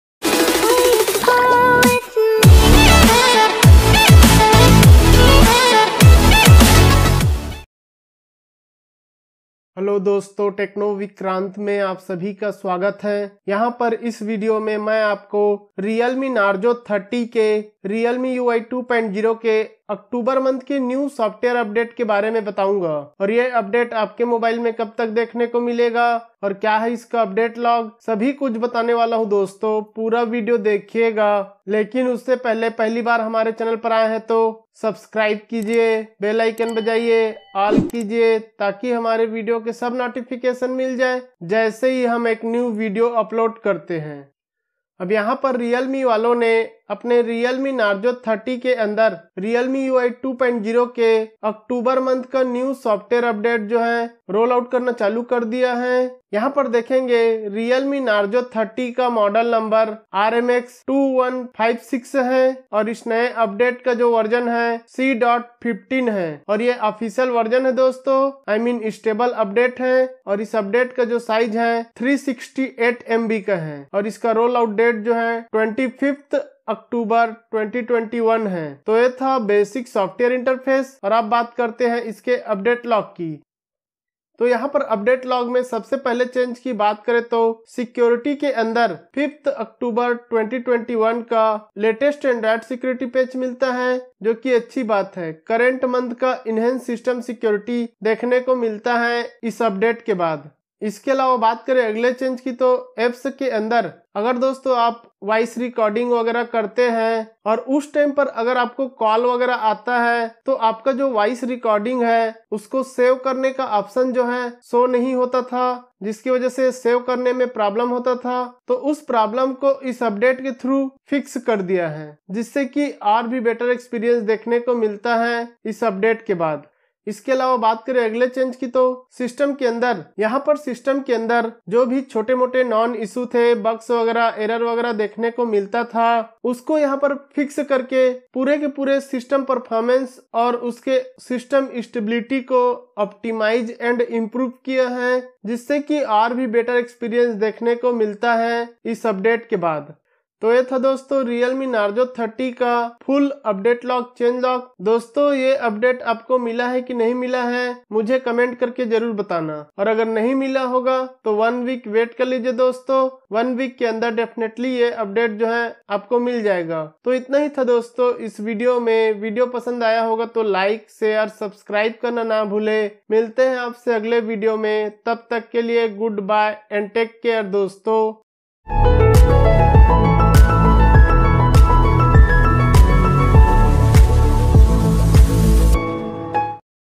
Realme Narzo 30 New C.15 Stable Update Full Review | Narzo 30 October C.15 Update | Hidden Features⚡
Dosto is video me maine aap logo ko Realme Narzo 30 ke Realme UI 2.0 ke October month ke Stable Software Update C.15 ke bare me bataya hu... October Stable Update C.15 me Realme UI 2.0 or Android 11 ke New Features add hua or kuch known bugs fixed kiye hai...Umeed hai aapko video pasand aayega